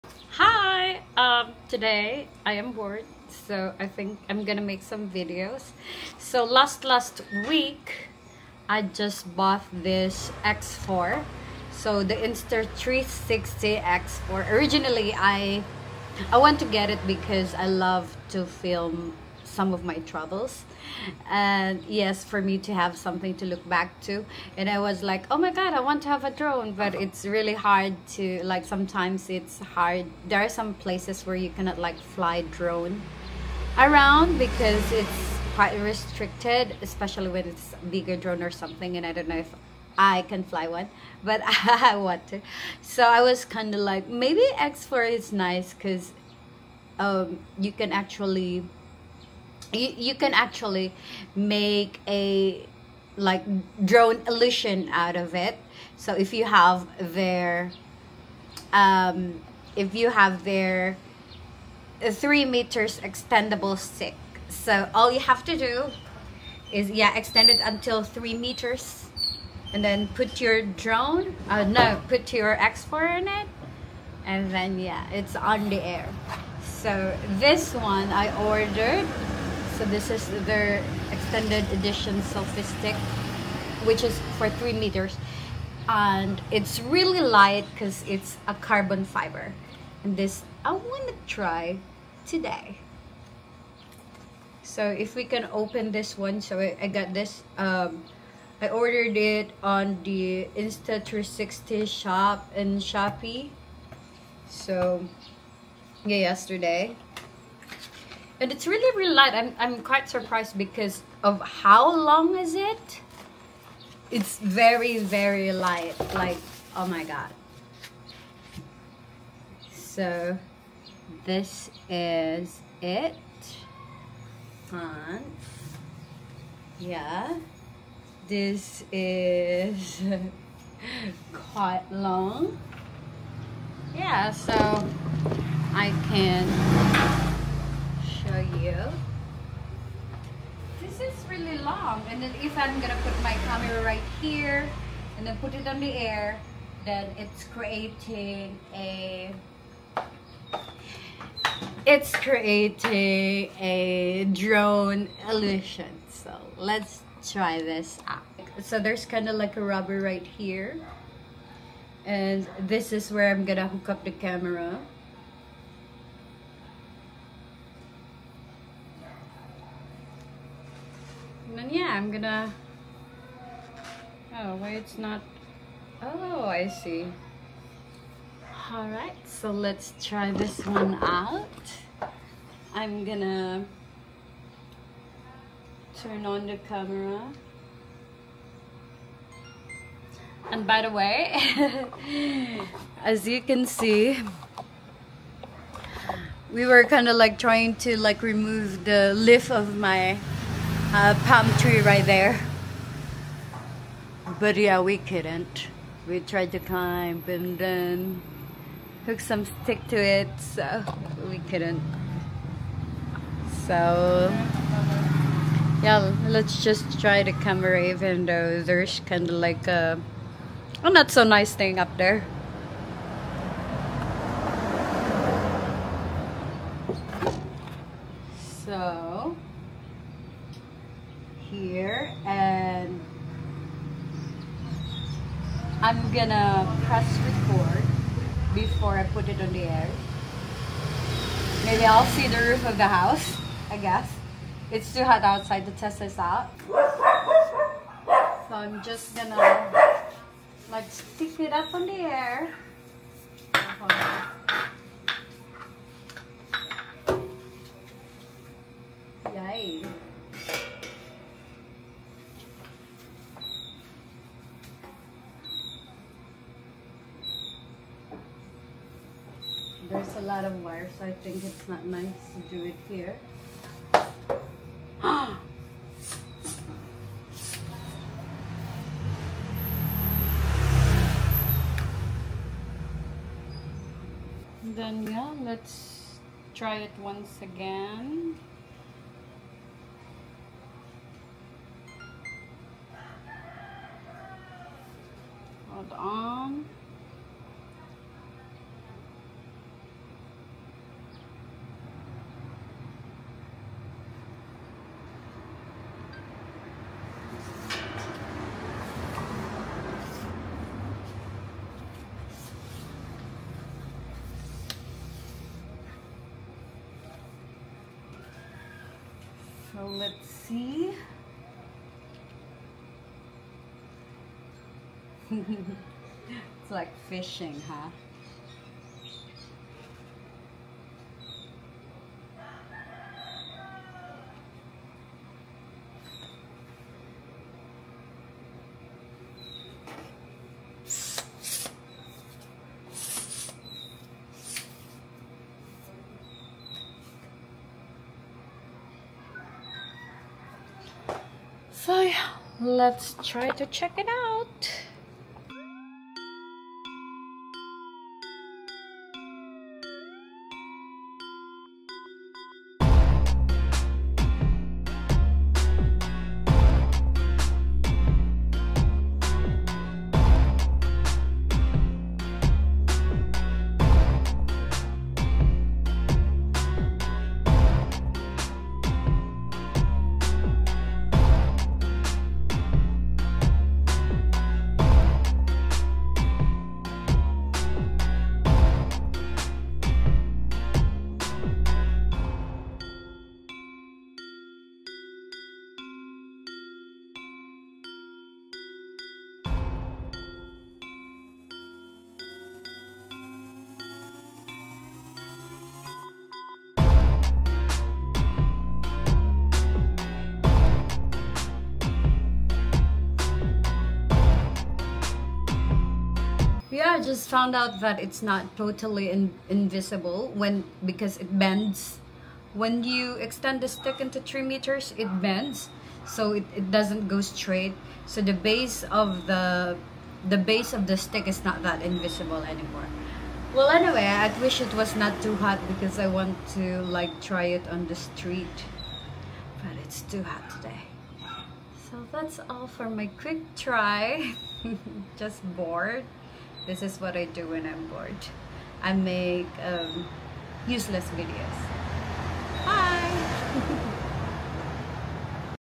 Insta 360 extended edition selfie stick to 3 meters
Trying out the the Insta 360 extended edition selfie stick to 3 meters. This is shot by DJI pocket 3.